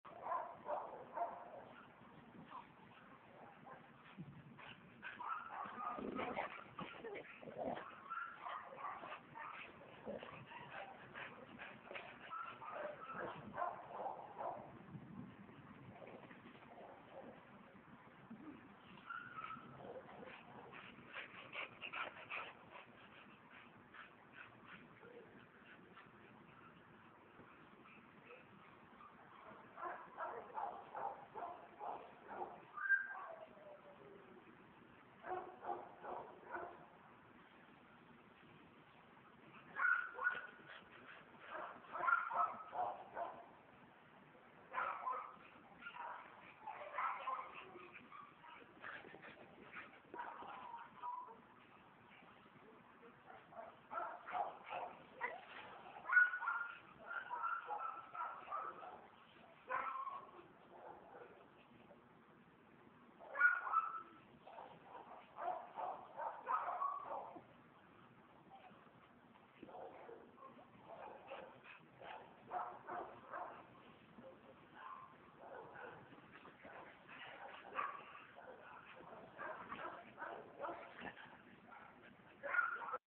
Video of adoptable pet named Mongo
Mongo is an adoptable pet with Ace of Hearts Dog Rescue in Beverly Hills CA. Please visit their